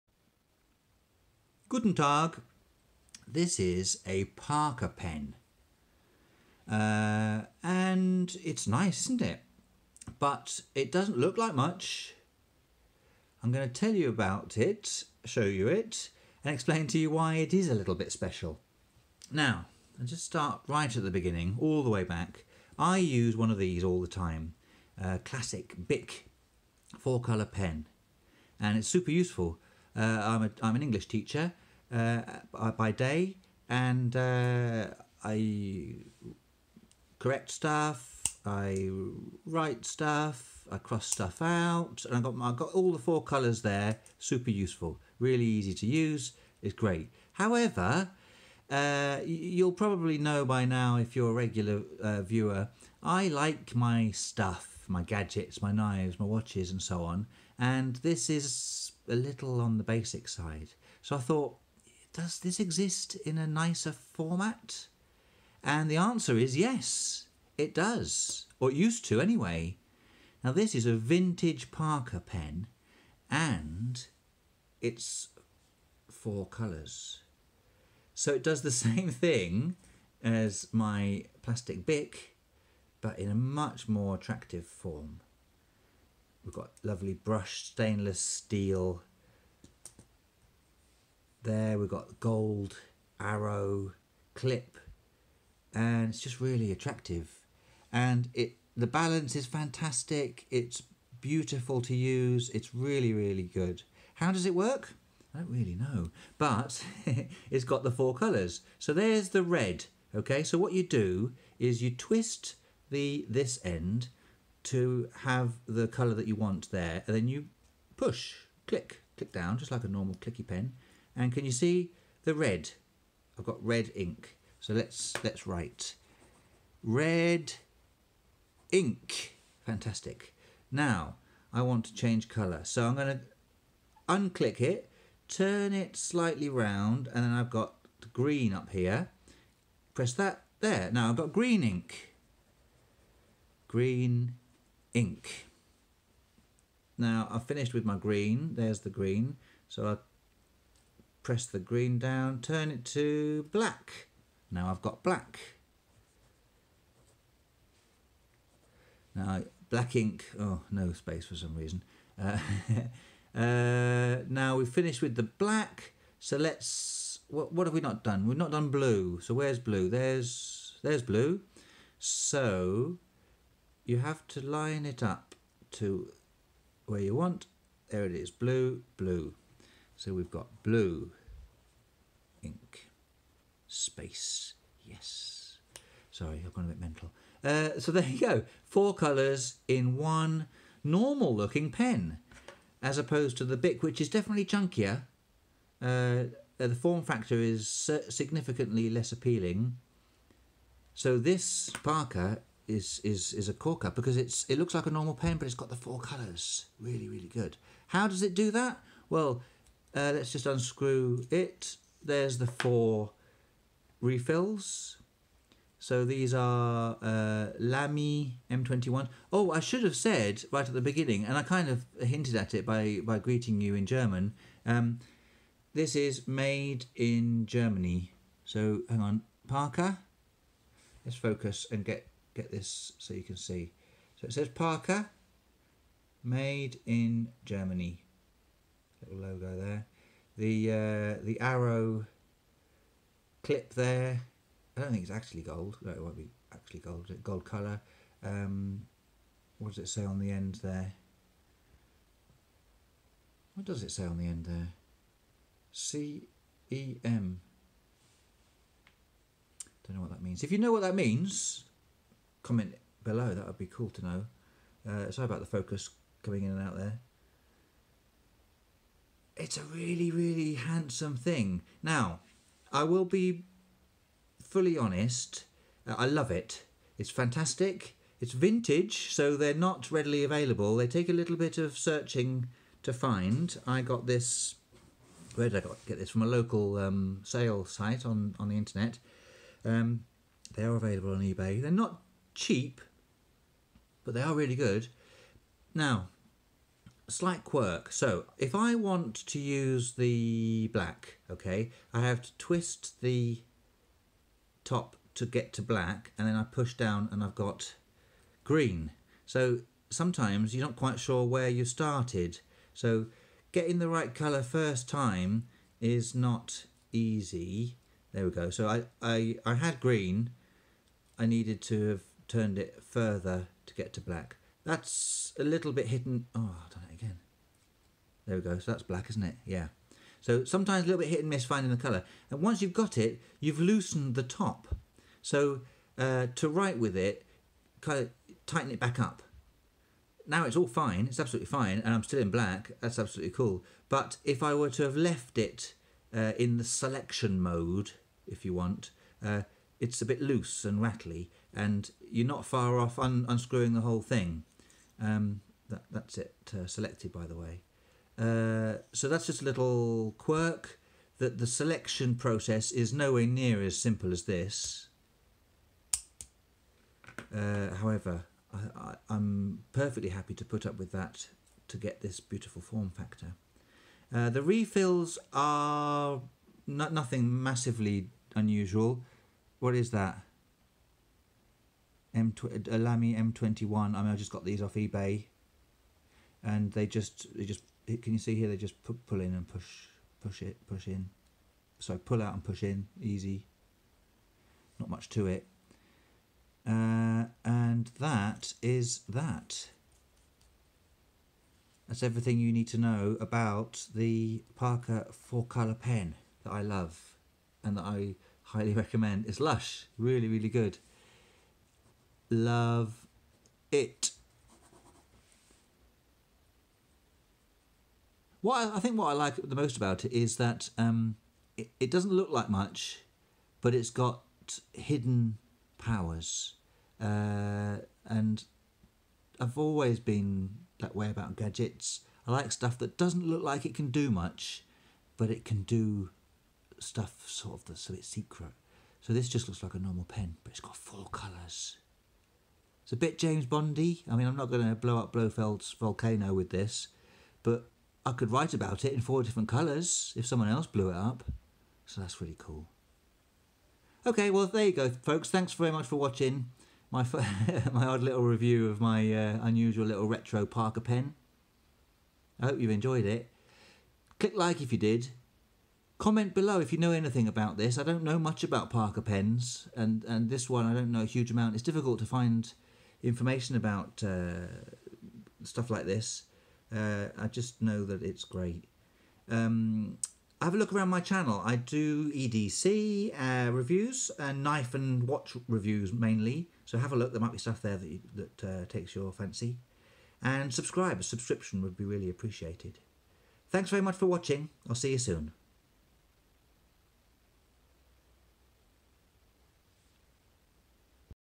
Parker Flighter de Luxe 4 colour vintage ballpoint pen - hands on review
A hands on review of the vintage Parker Flighter de Luxe 4 colour ballpoint pen.